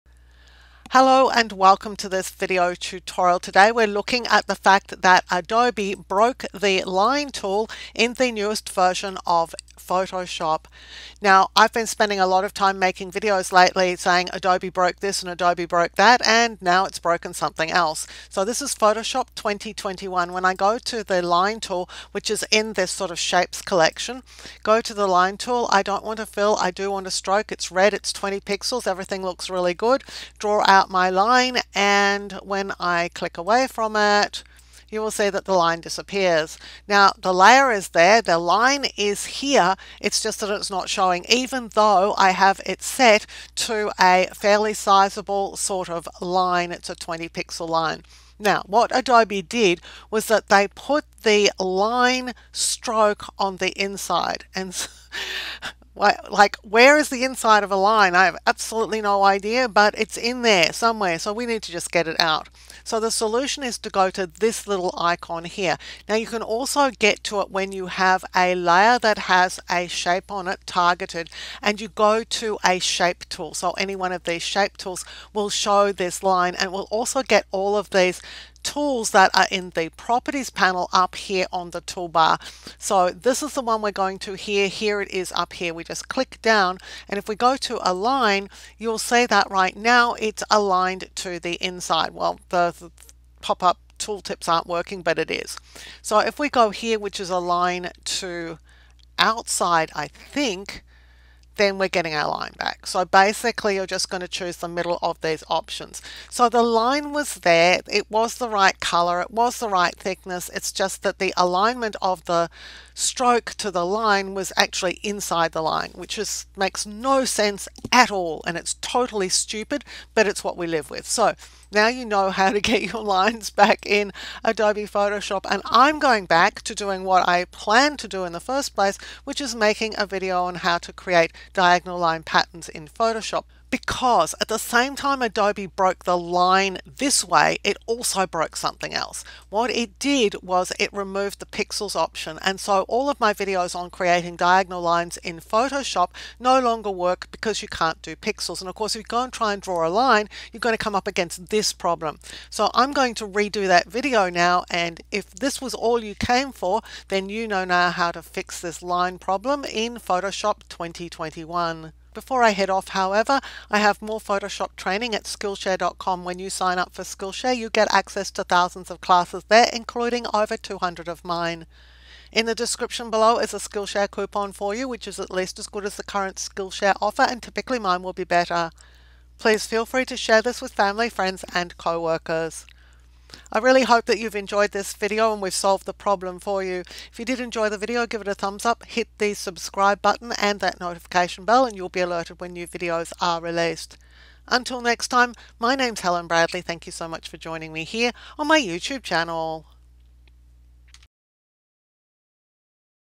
Photoshop 2021 - Line Tool Broken - Quick and easy fix! SOLVED
Adobe changed the line tool behaviour in Photoshop 2021 so it doesn't work like it used to work. Now when you draw a line with a stroke around it, you can't see the stroke, and when you move away from the line it disappears. I'll explain the problem and the solution which is easy to do but far from obvious.